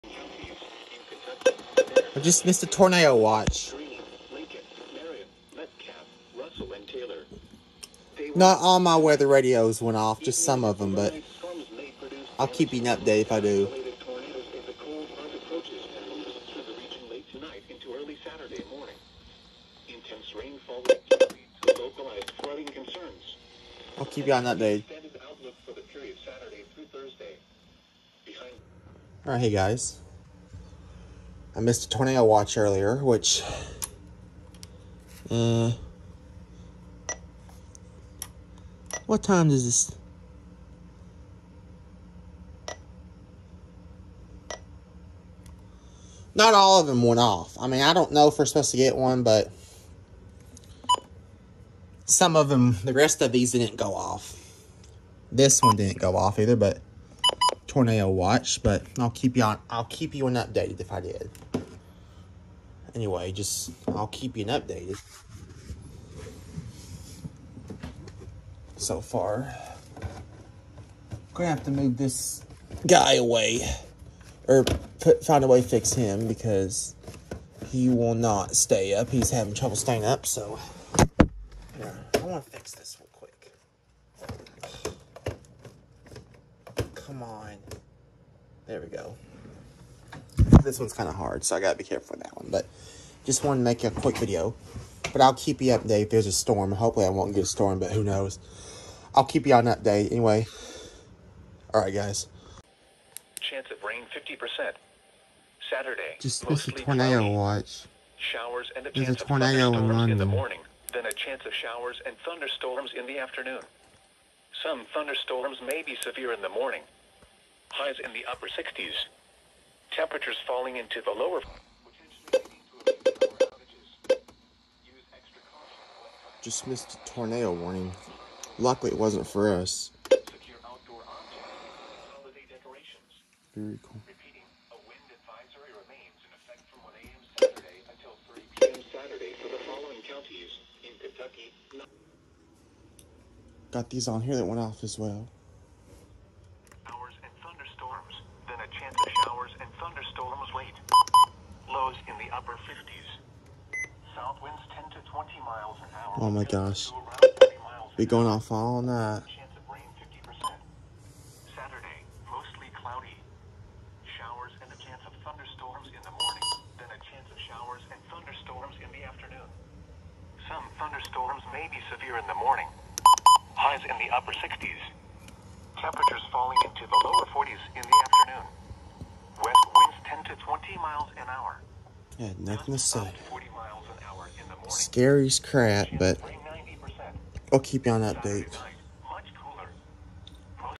NOAA Weather Radio Missed EAS Tornado Watch/Warning Severe Thunderstorm/ Flash Flood Warning.
O Yes We Had A Pretty Bad Storm And Yes I Couldn’t Sleep At All Last Night. Because Those Thing We’re Going Off Like Crap. And The Reason I Didn’t Video The EAS Was Because I Was In Bed And Tired And If I Stayed Up All Night I Would Have Been Pretty Freaking Tired So That Why I Didn’t Want To Video The EAS Because I Wanted To Sleep And Be Prepared In The Event Of A Tornado. That Why. ￼